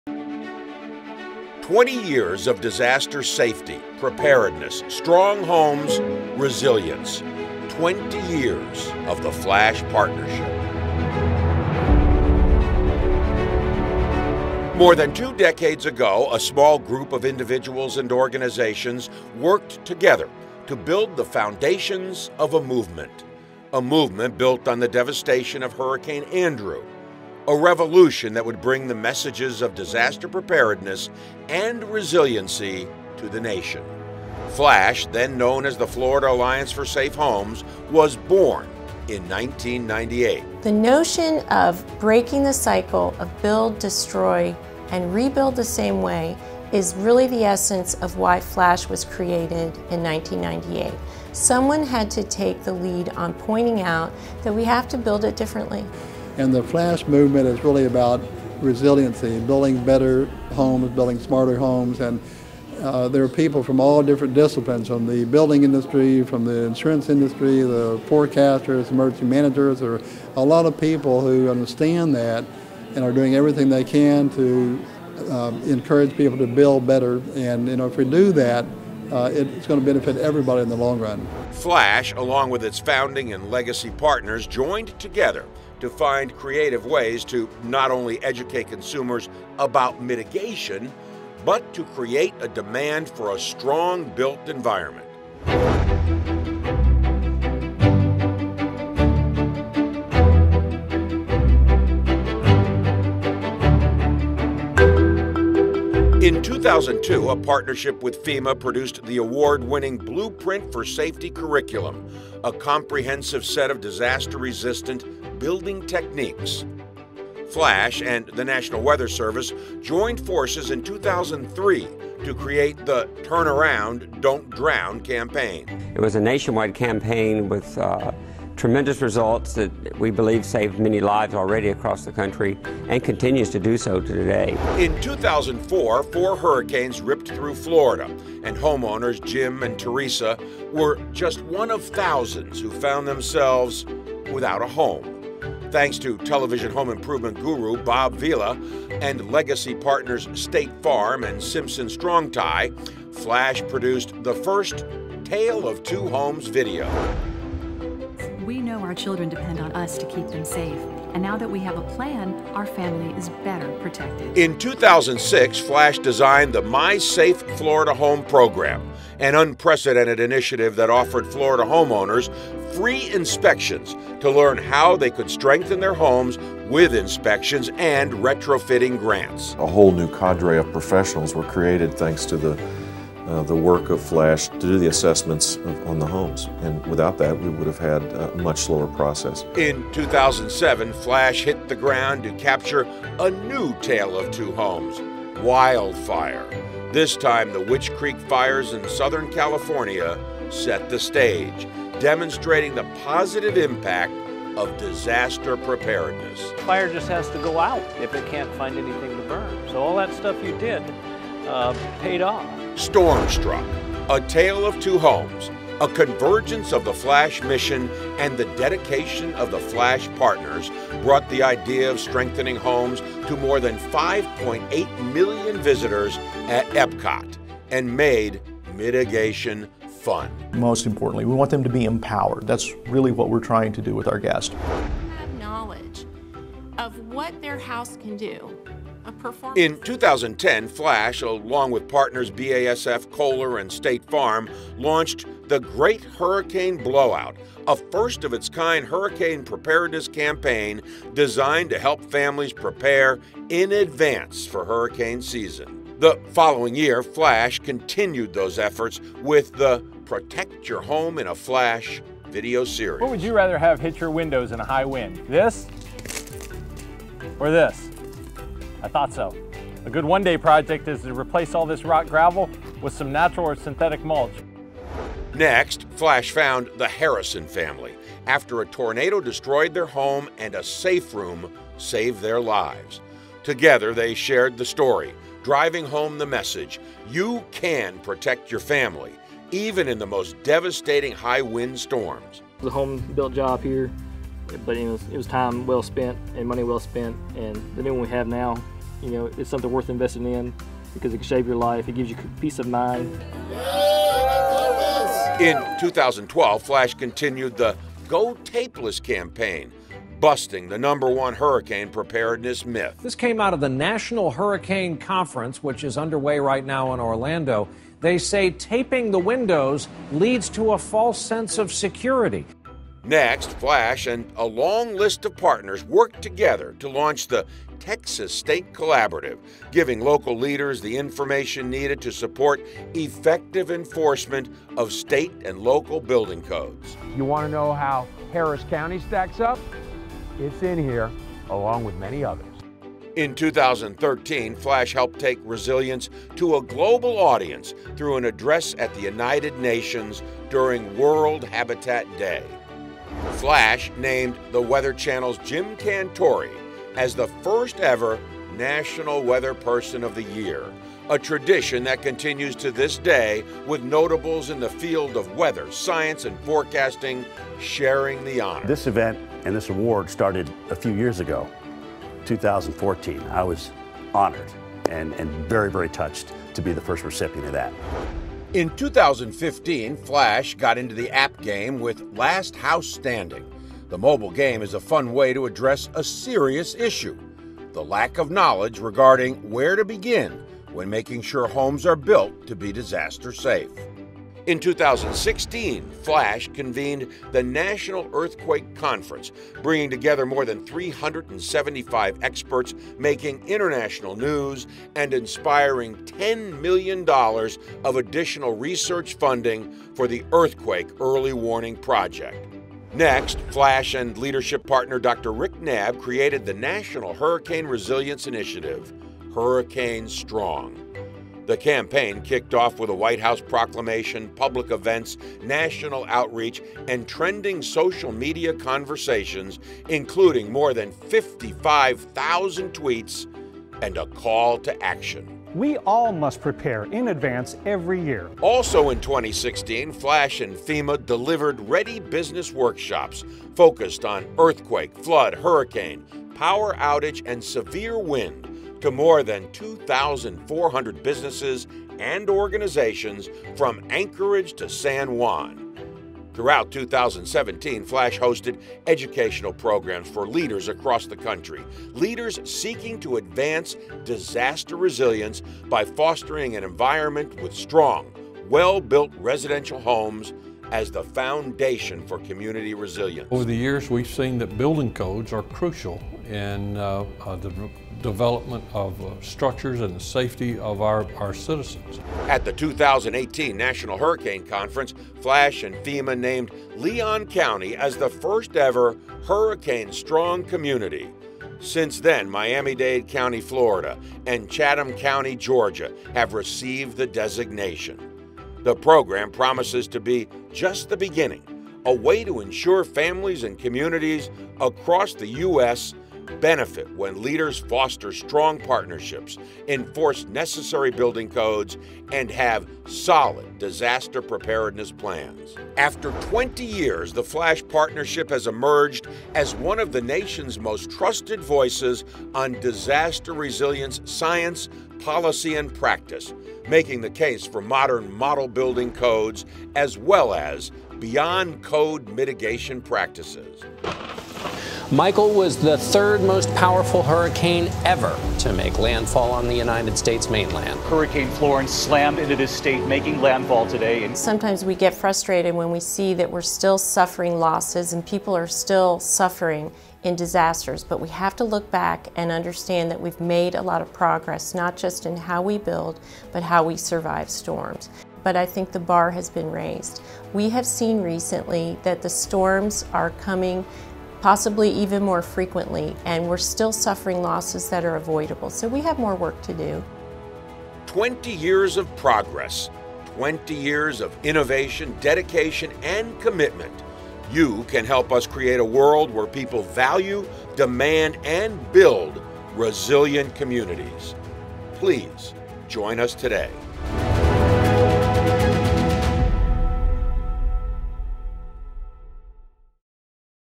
Federal Alliance for Safe Homes - FLASH 20 Year Anniversary
A look back at 20 years of disaster safety and the FLASH Partnership.